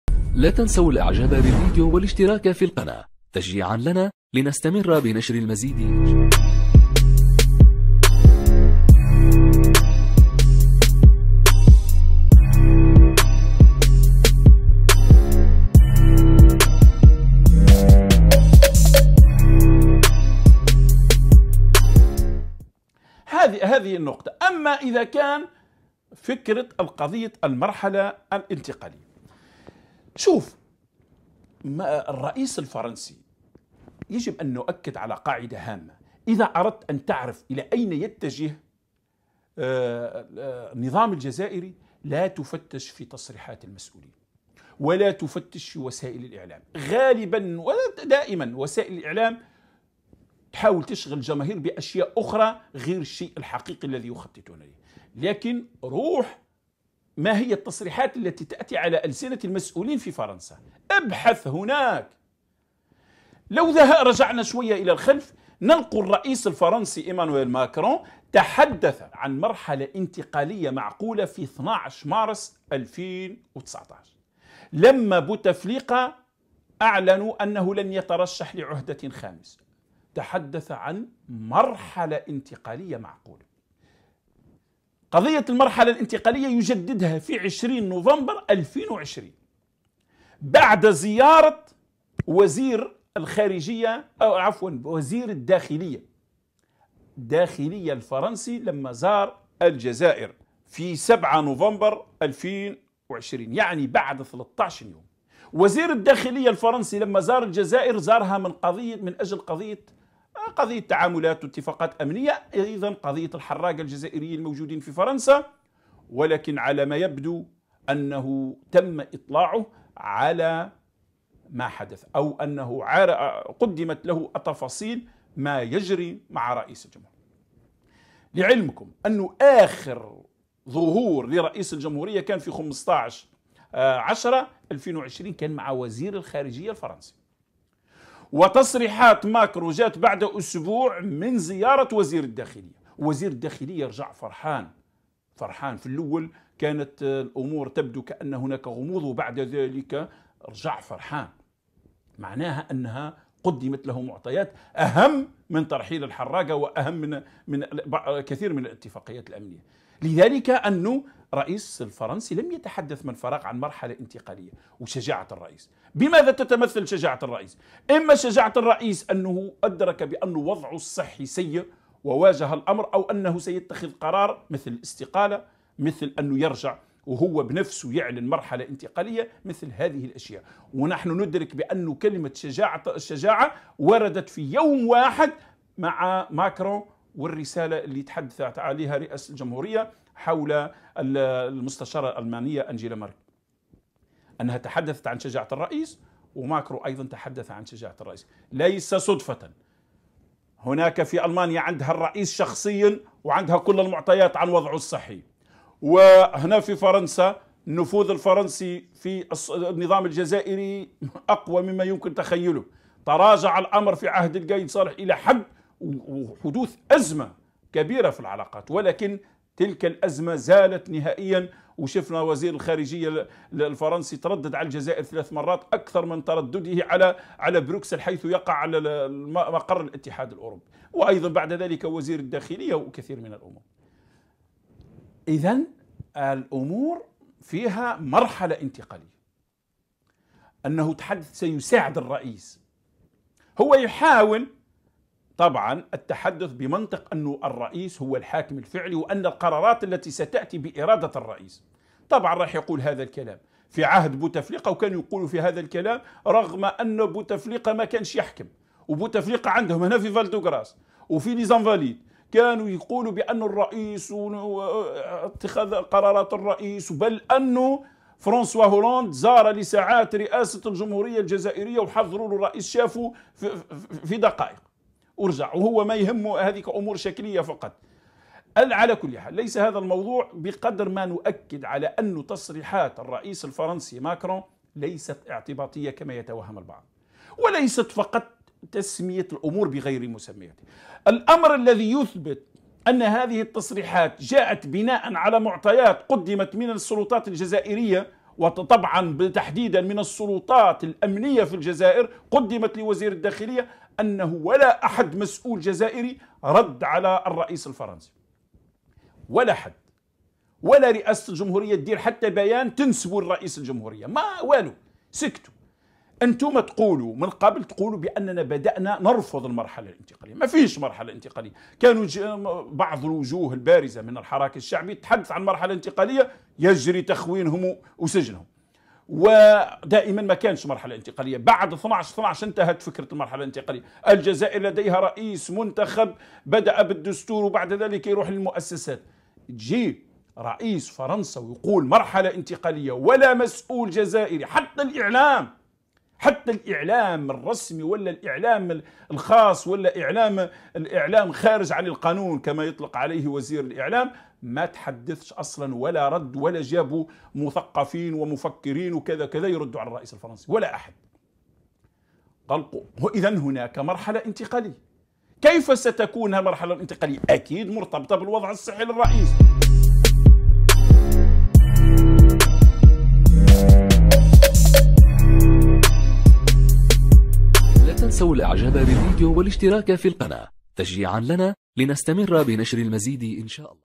ماكرون أعلن عن مرحلة انتقالية بعد عودة وزير داخليته من لقاءات مع جهات أمنية في الجزائر
من يرغب في التواصل معنا:
مواقعنا على شبكات التواصل الاجتماعي:
تويتر: